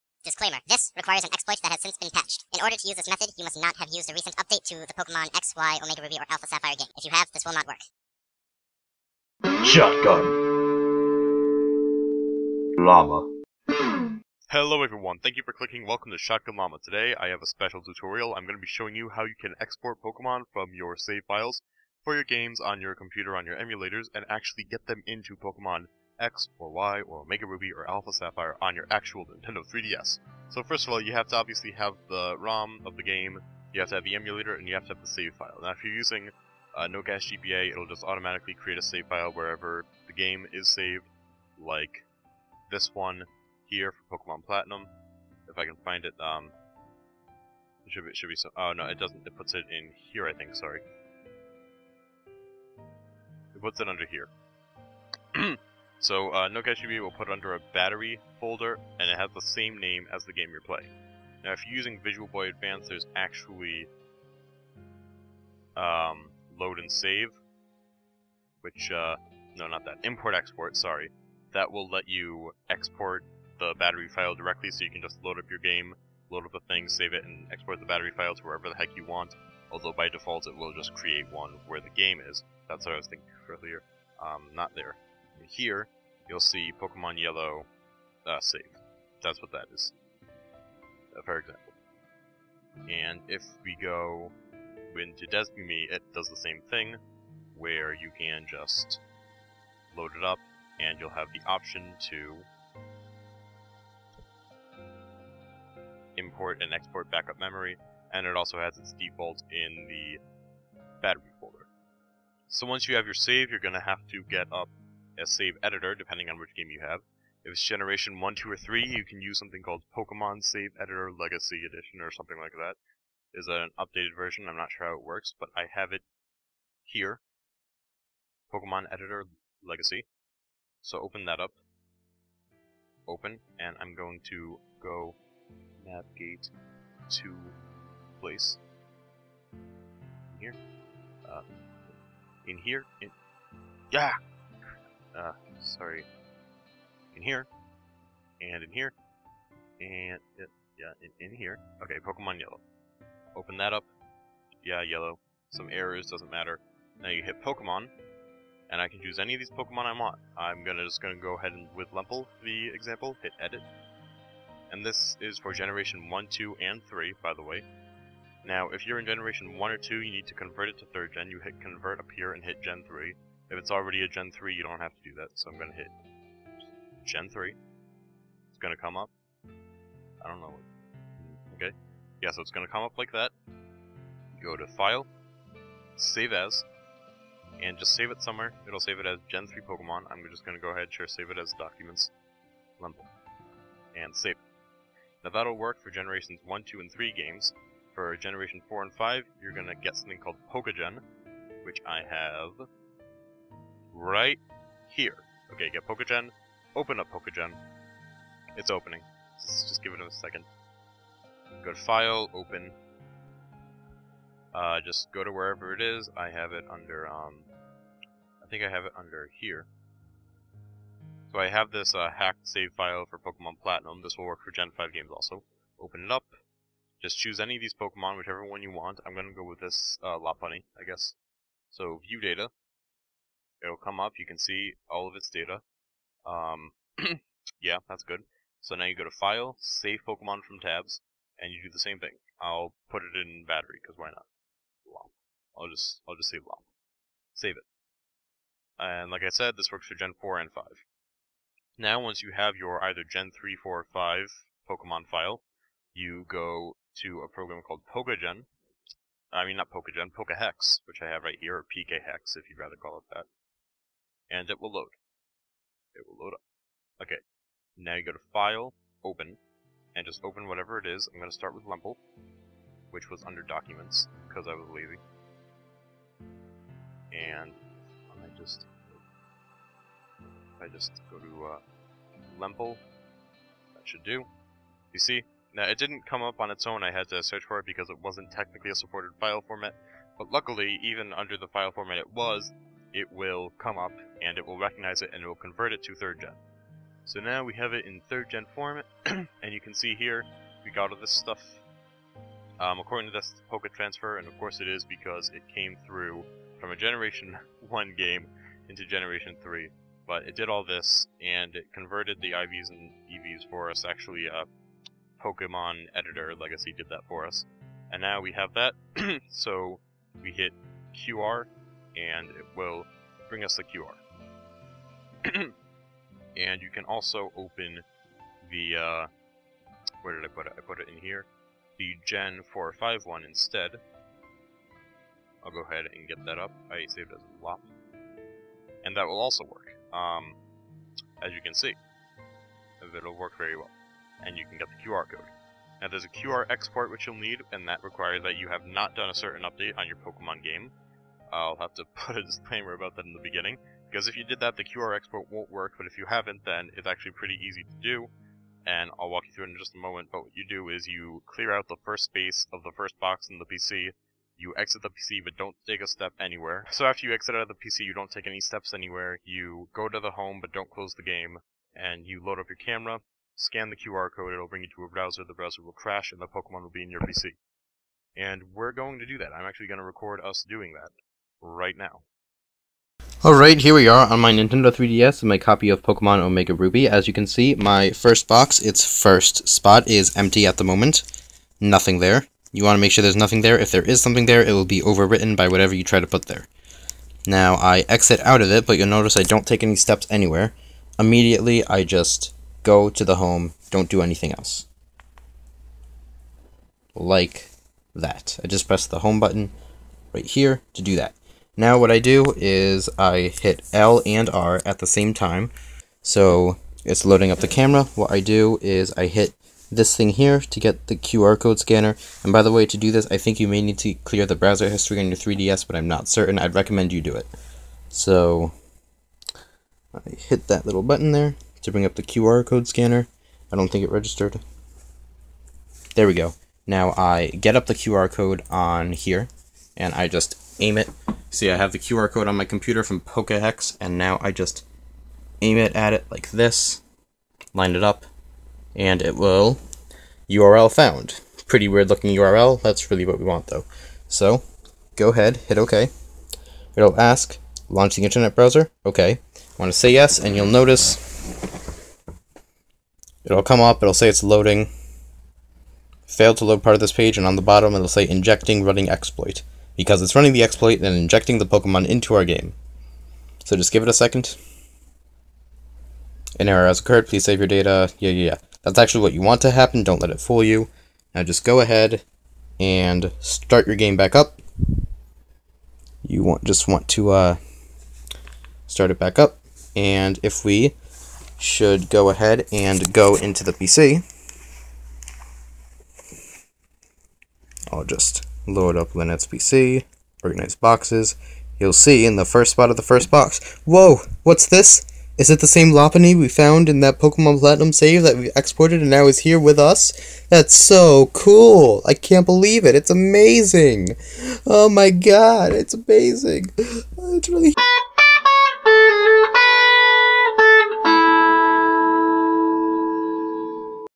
How to Export Pokémon From Emulator
In this video, I show you how you can export your Pokémon companions from games on an emulator to a 6th gen game on a physical 3ds system! So long as you've not applied a recent update, it should be very easy to do!

Steps for Gen I-III:
Locate your battery file.
Open it using Pokémon Editor Legacy. Select the game it's for.
Hit "Pokémon", then navigate to the Pokémon you want to export.
If it's from a Gen I or II game, you must click the "Convert" menu and Select "Gen III".
Then save your Pokémon somewhere to your computer.
Continue to "Universal Steps" below.

Steps for Gen IV-V:
Open it using PokéGen.
Select your Pokémon and choose "View Data".
Hit "Save from Tabs" from the File menu and save your 'mon to your computer.
Continue below with "Universal Steps".

Universal Steps:
Open your Pokémon in PKHex, and hit "QR" in the corner.
Go into your 6th gen game, and make sure the 1st slot of the 1st box in the PC is empty, exit the PC but don't go anywhere.
Without closing the software, go to Home by pressing the Home button.
Hit L + R at the same time to open up the camera, and hit the QR code button.
Scan the generated QR code from PKHex and choose to go the URL.
After a little while, the webpage will crash, and you should reopen your game to find that spot 1 of box 1 in the PC is now occupied by your Pokémon!

Background music from purple-planet.com.